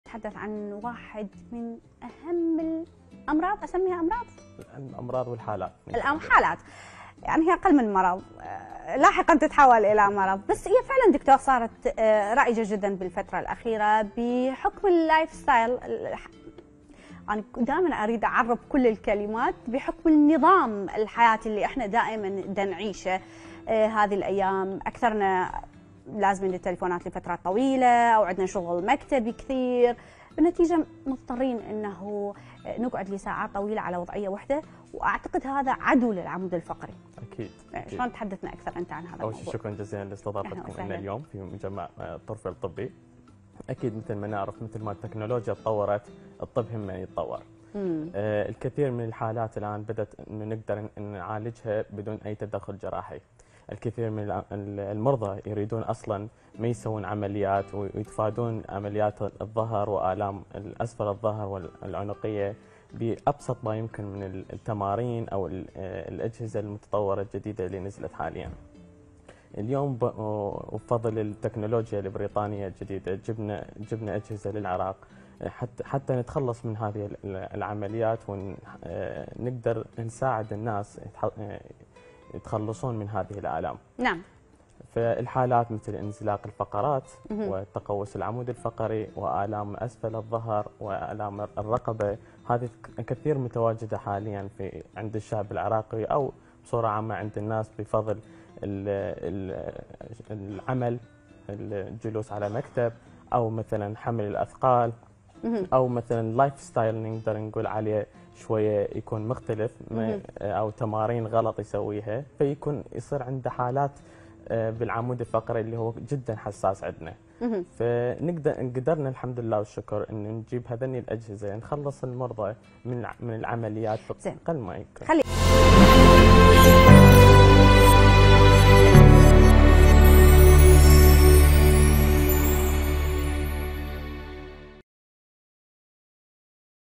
أحدث الأجهزة الطبية لعلاج العظام والعضلات وتجنب العمليات الجراحية مع الطبيب د. محمد إبراهيم
11258📺 افقي